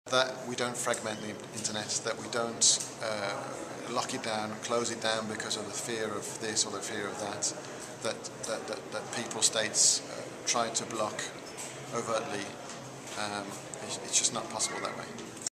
IGF11 Lee Hibbard talks about his hope for the future of the Internet
Lee Hibbard of the Media and Information Society Division of the Council of Europe responds to the question "What is your greatest hope for the future of the Internet?" Recorded by a documentary team from the Imagining the Internet Center at Elon University as part of a video survey of nearly 80 of the more than 1,000 participants in the 2011 Global Internet Governance Forum in Nairobi, Kenya.